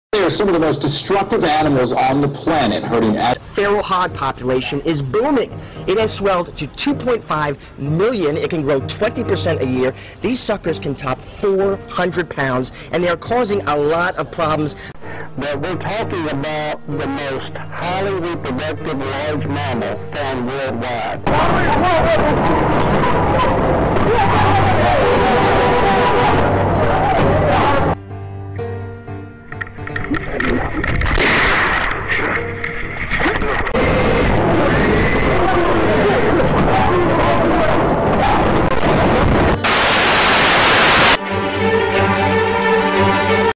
The WALKING PIG (editted trailer)
Promo of an upcoming parody of the walking dead